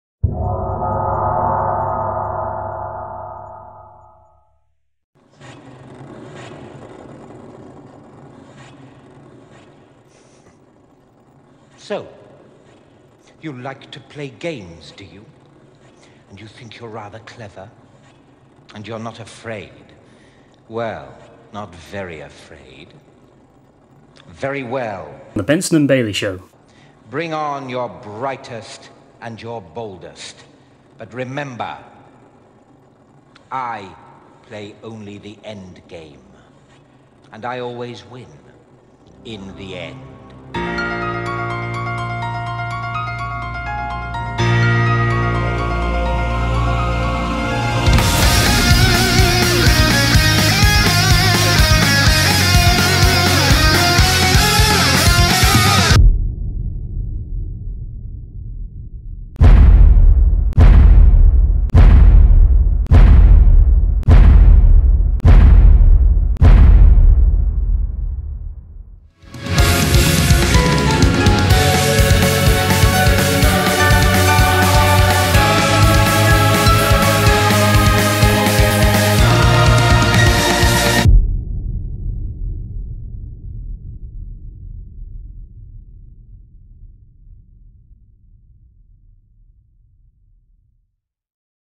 So... you like to play games, do you?
Join Benson & Bailey as they explore a collection of horror games, every night, for the rest of October... it's FRIGHT NIGHTS!